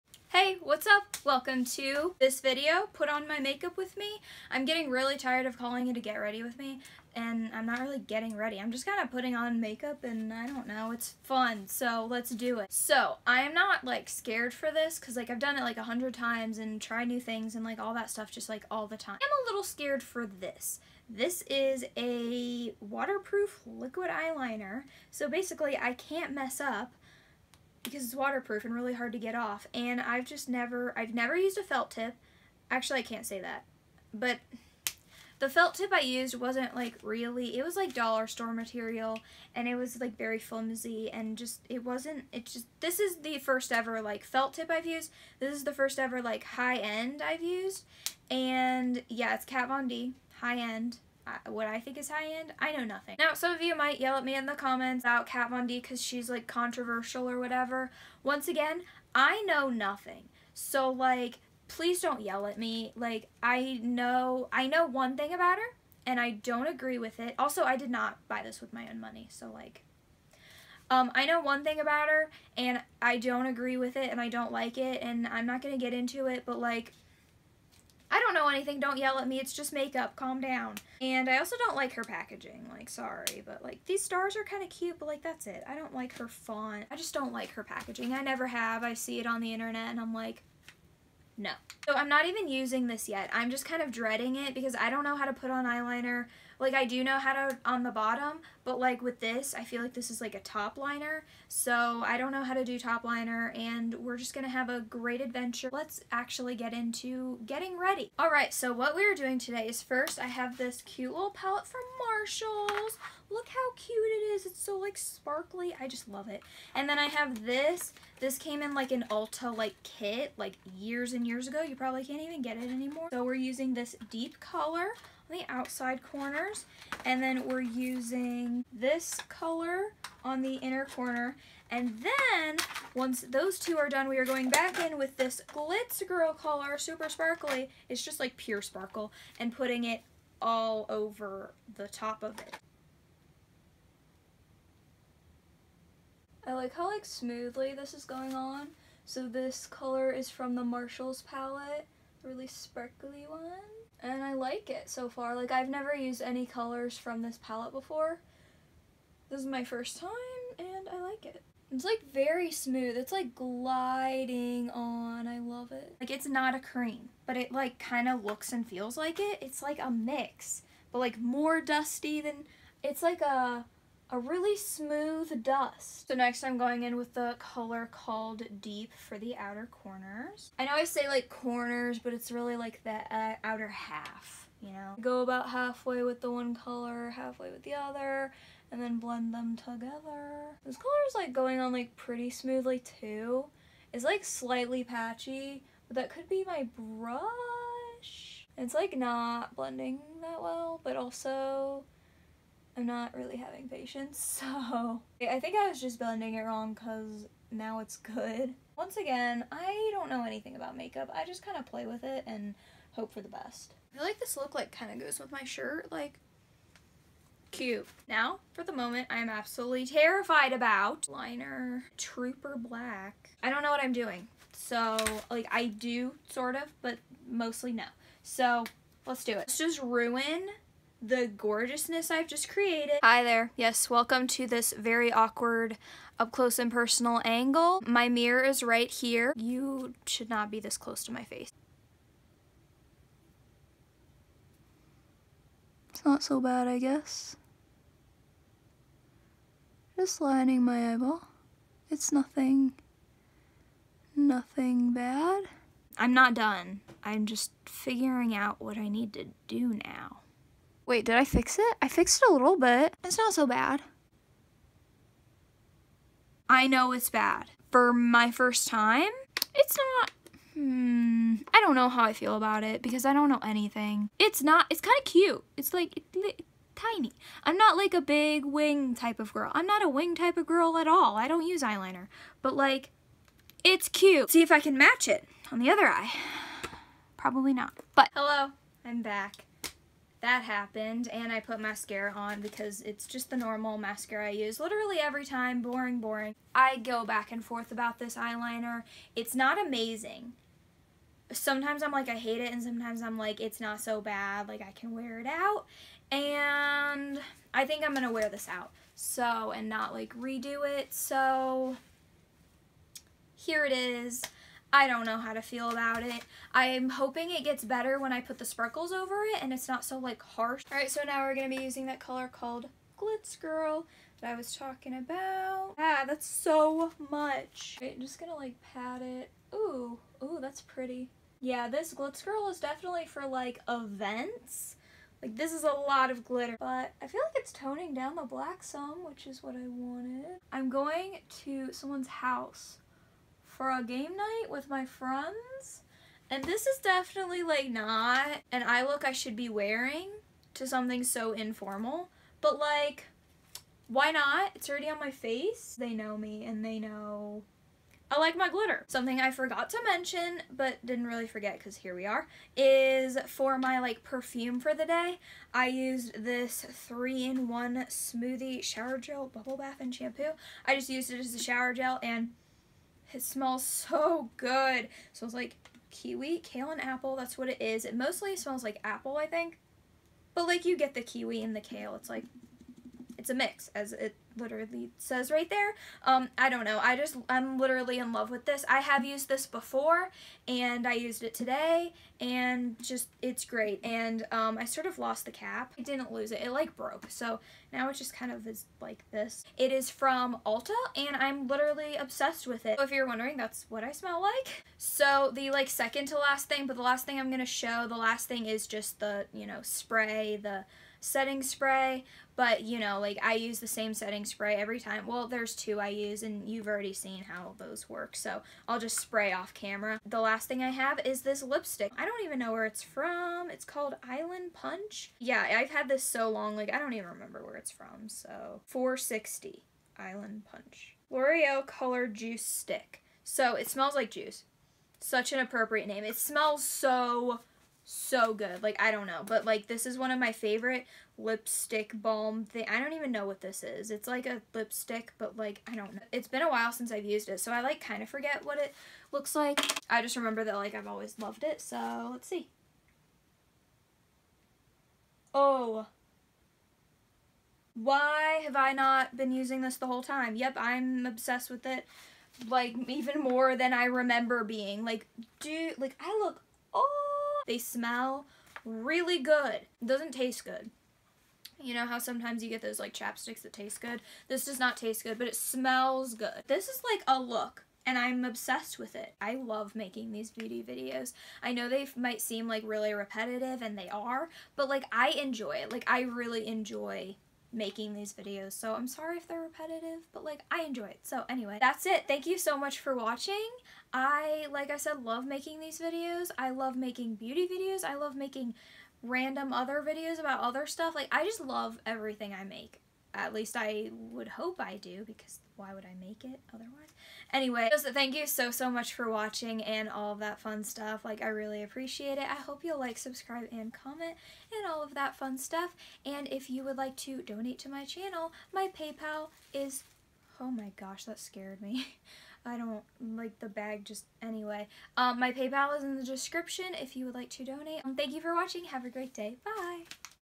Doing My Makeup With a New Kat Von D Eyeliner and Other New Products
This is just a little get ready with me/ doing my makeup for a video and me using a felt tip eyeliner which I never use, so the experience is quite interesting lol.

I read all my comments and would love to hear what you have to say!